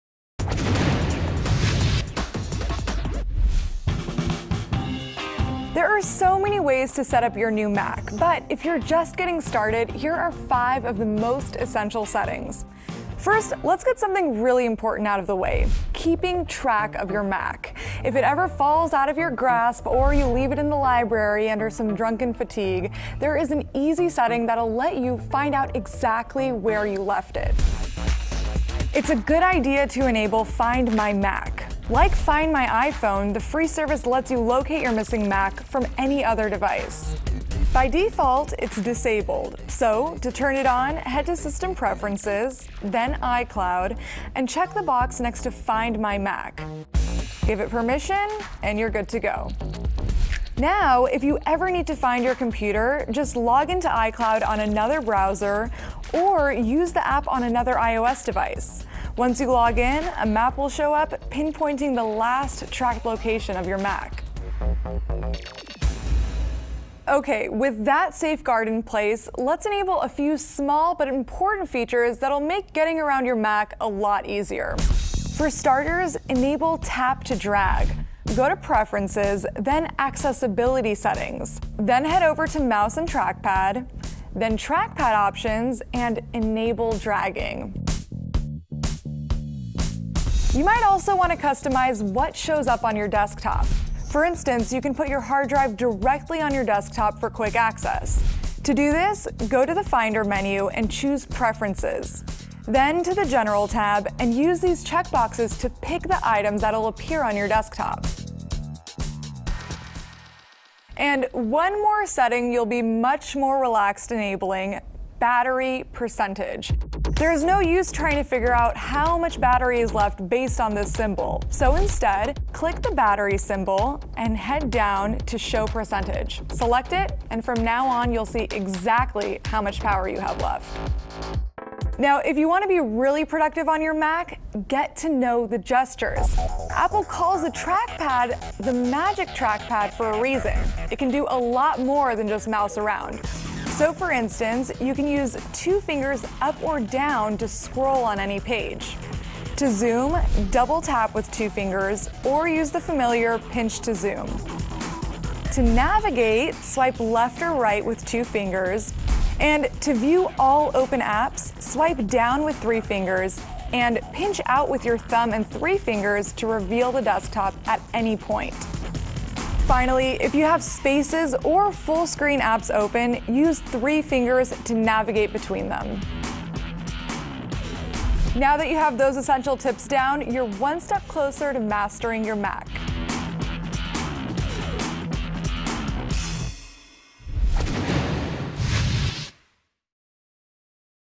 The Fix - Five essential tips on setting up your MacBook
These customized settings will get you up and running so you can get the most out of your MacBook.